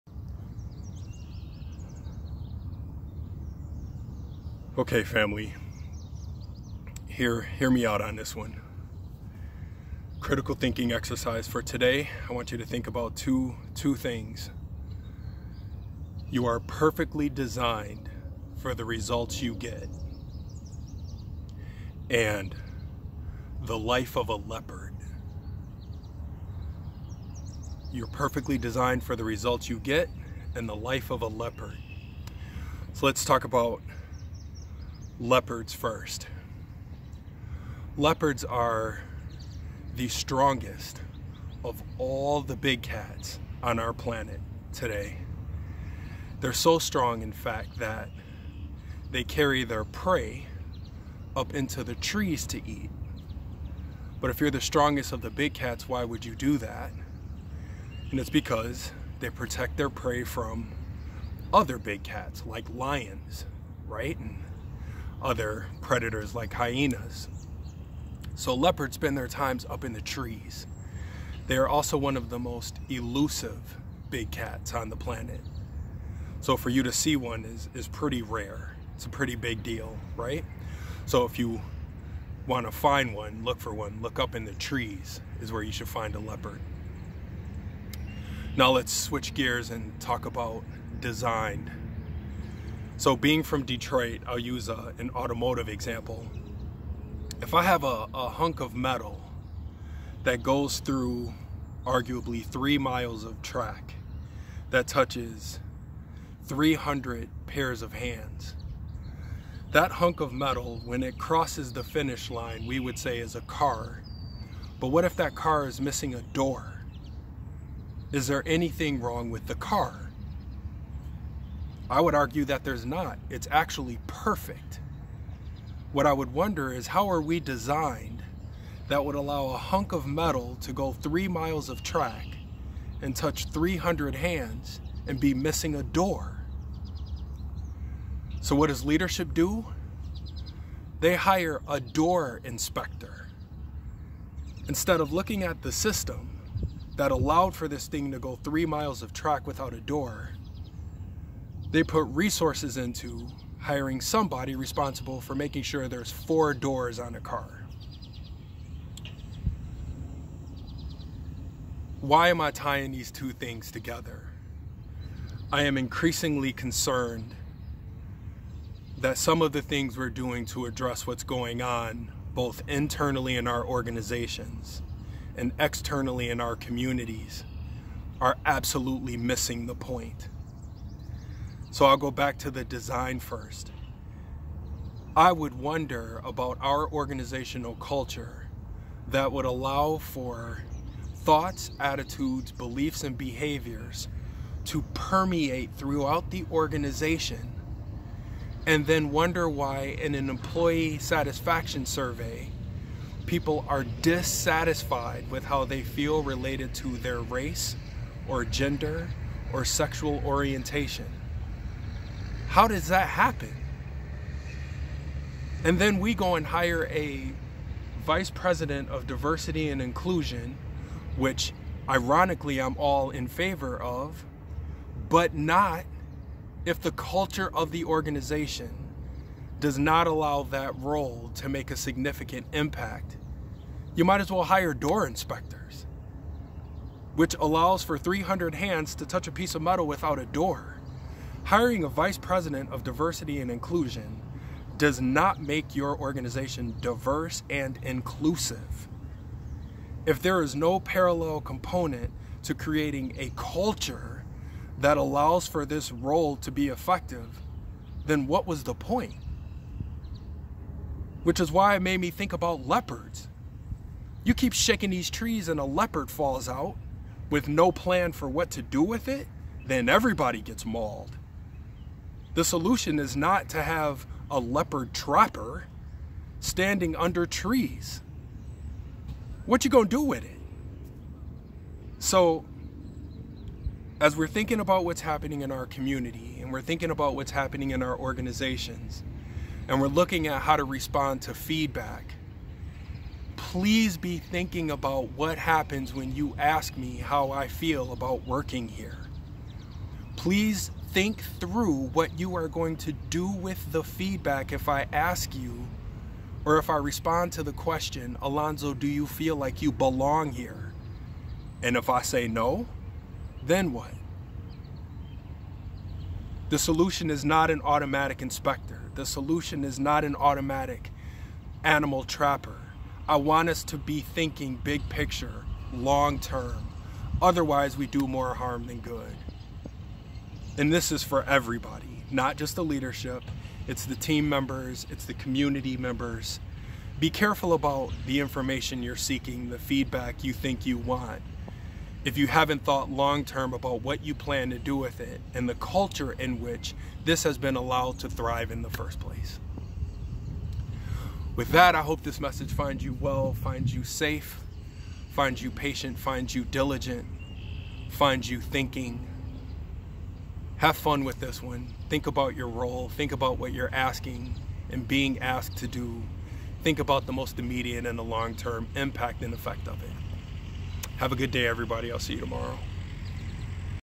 Reckless Response To Feedback
As the saying goes, 'If you keep shaking trees eventually a leopard will fall out'.  I am all in favor of seeking feedback.  No plan on what to do with it may get you mauled.